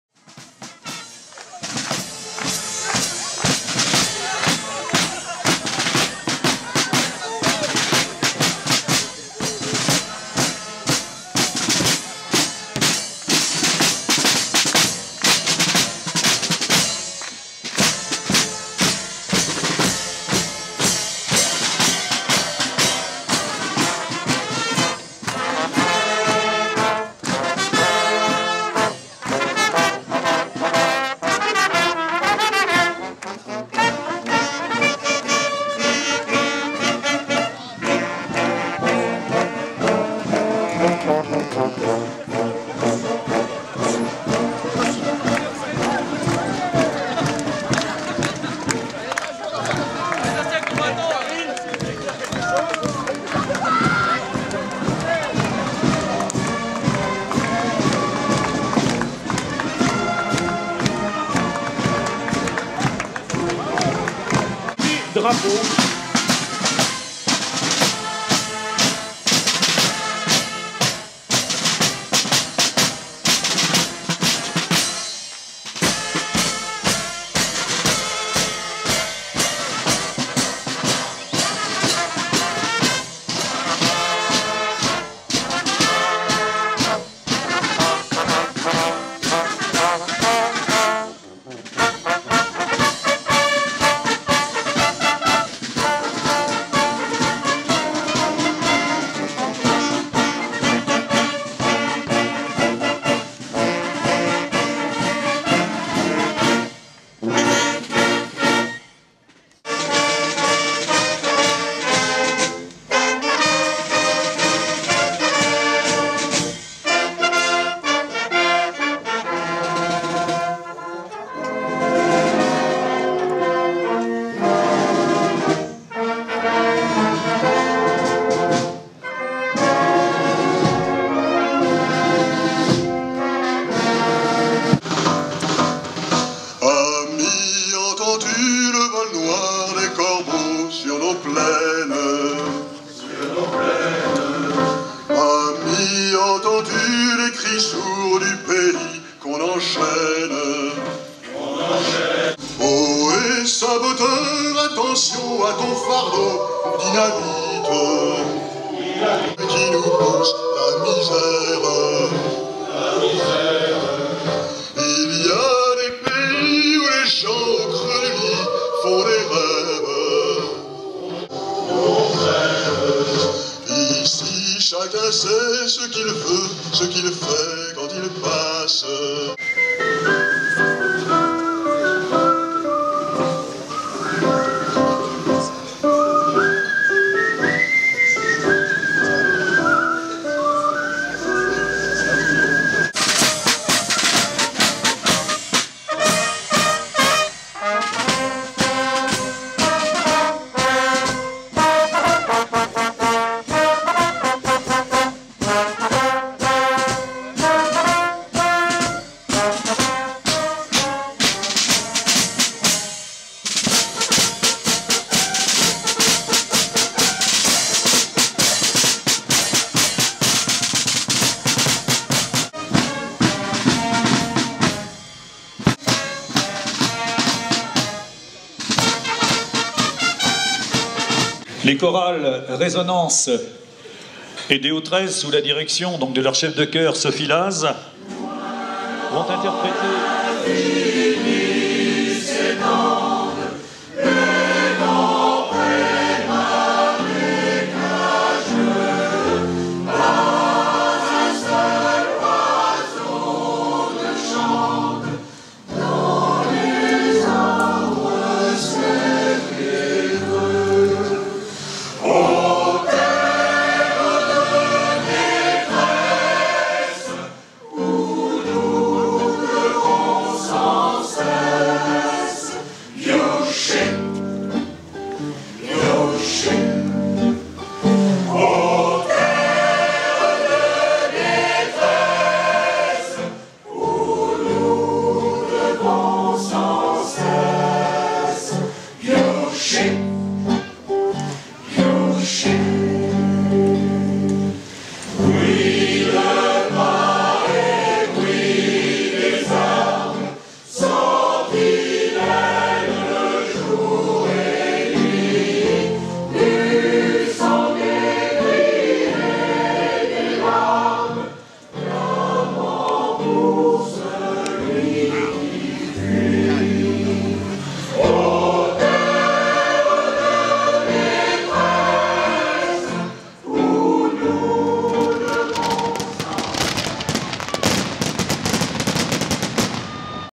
Vidéo sur l'île de ré la Flotte 8 mai 2010
Ile de Ré. la flotte défilé du 8 mai 2010 -  le 8 mai 2010 - harmonie de la Flotte - ile de Ré
chorales rétaises